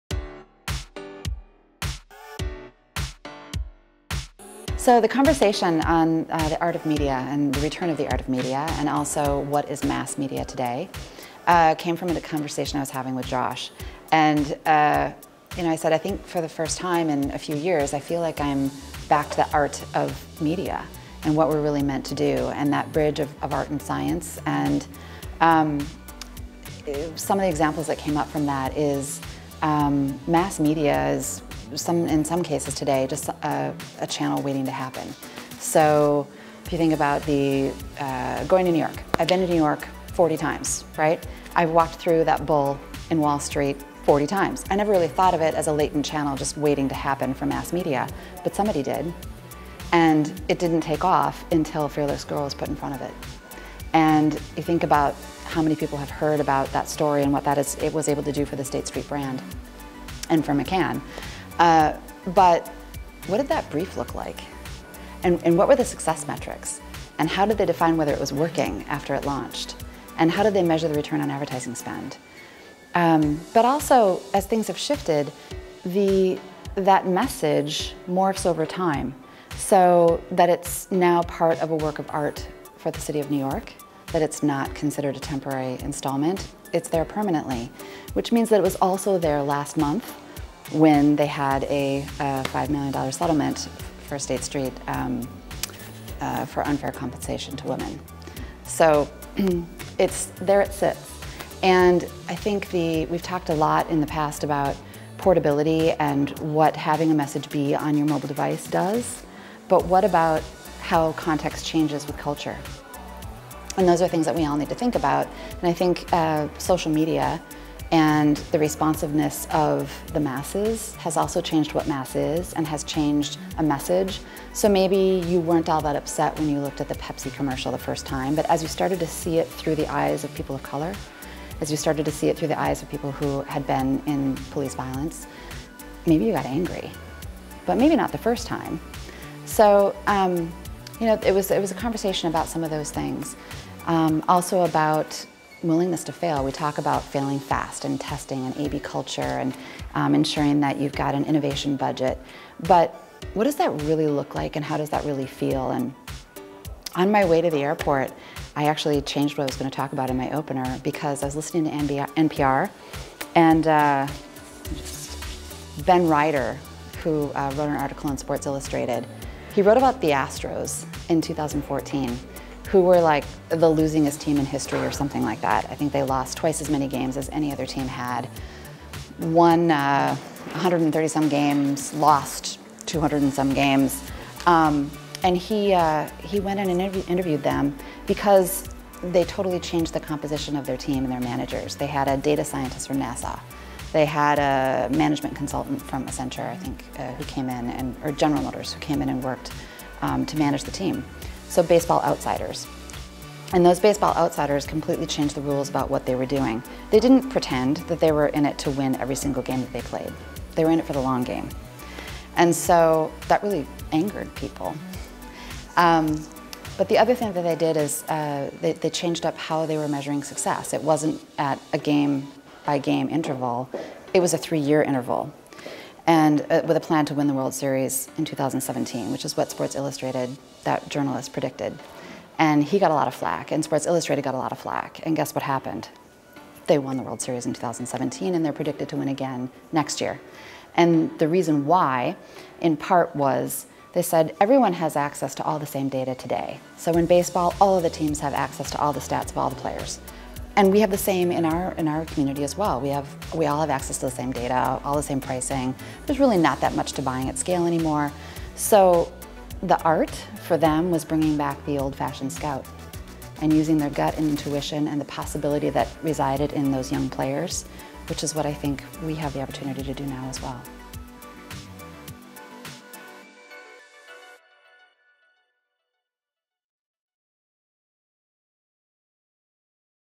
Aimee Reker, Managing Partner at Leveret Marketing
Leveret Marketing’s Aimee Reker shares her views on the return of the art of media and the importance of bridging art and science. She shares how we all focus internally on things like willingness to fail, the importance of testing AB culture or insuring you have an innovation budget, but how does that look and feel?

She digs more into how context ultimately changes with culture. Social media greatly impacts mass media is and ultimately has the power to change the message. Check out her examples here.